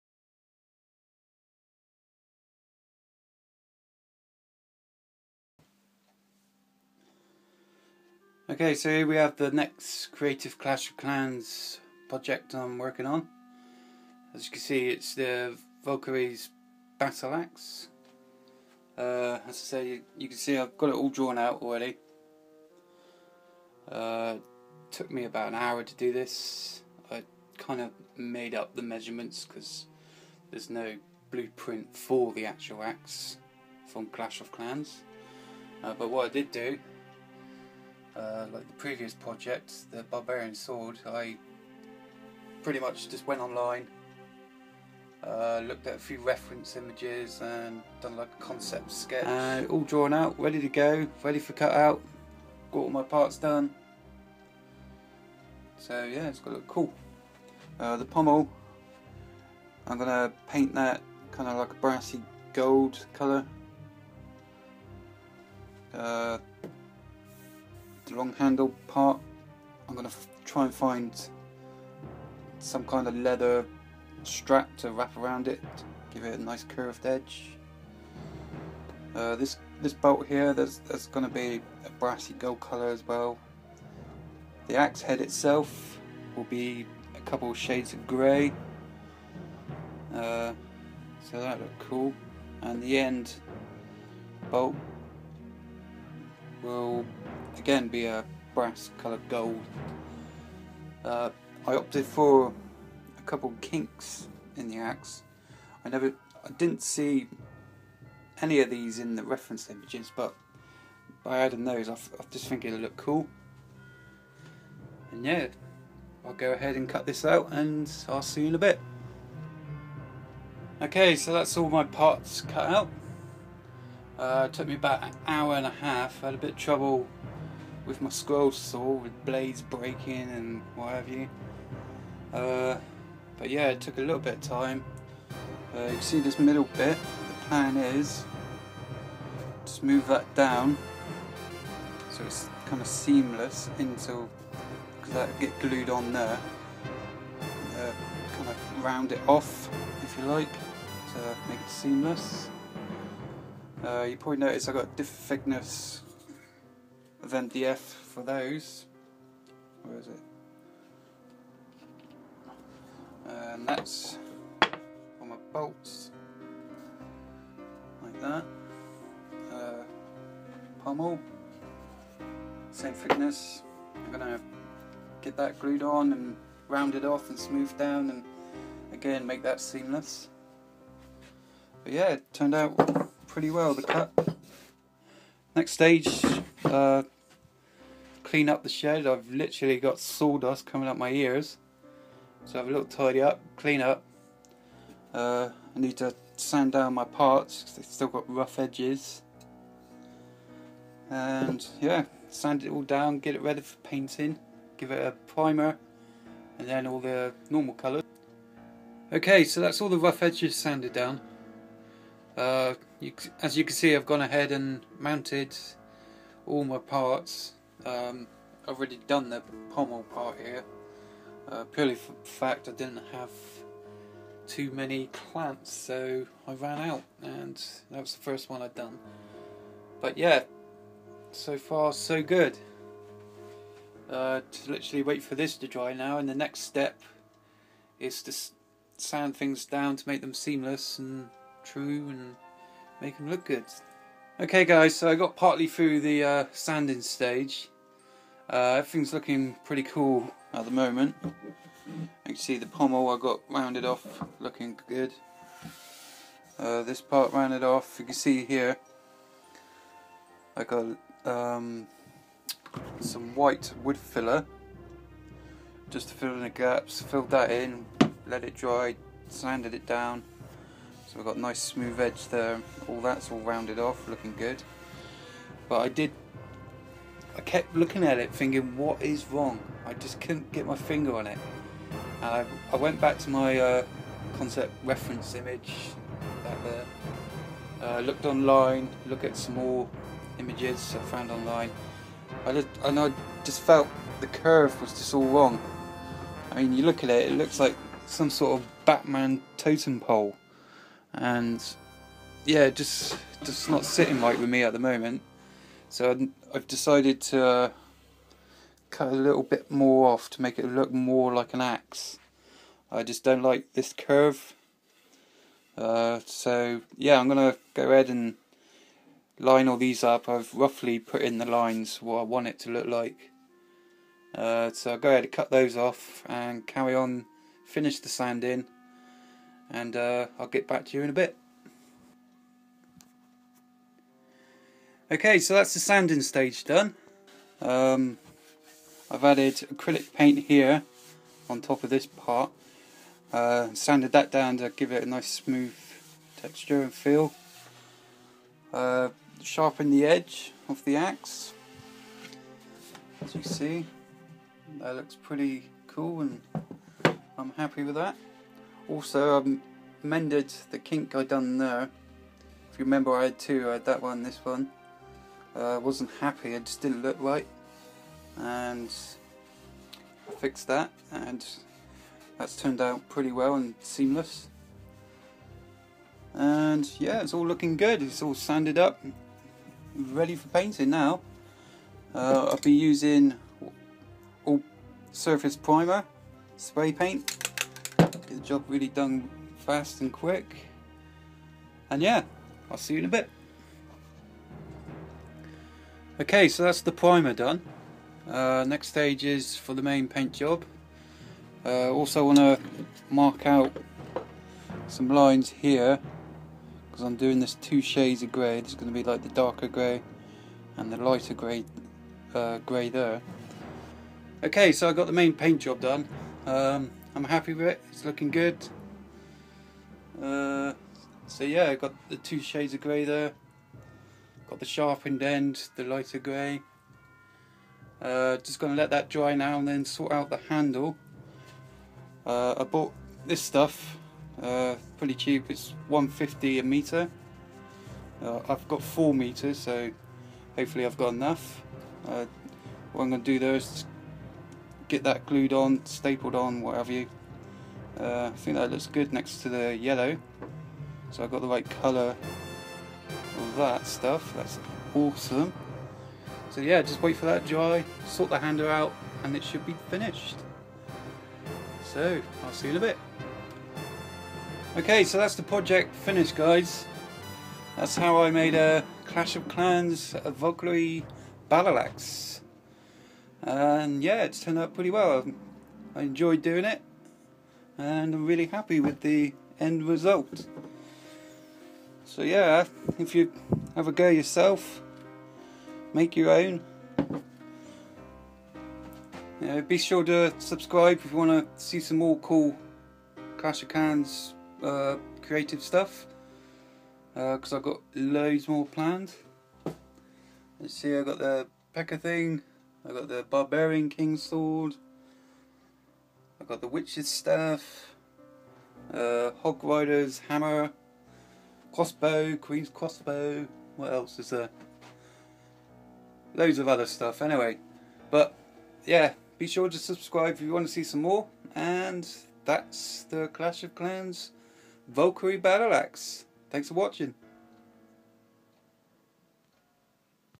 How to Create a Clash of Clans Valkyrie Axe
Here's my second creative Clash of Clans project.  How to make your own CoC Valkyrie battle axe using MDF wood, white primer, acrylic paint and a fake leather strap.

This would be great for anyone planning a Clash of Clans Valkyrie cosplay and needs the axe prop or just a keep sake.

Step by step blog post - coming soon...